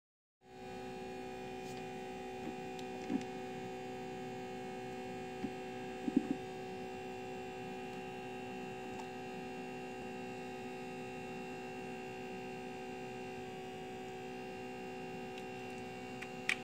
trickle
Removed some coolant to see just how much if any coolant is being pumped from the return line, its just a trickle.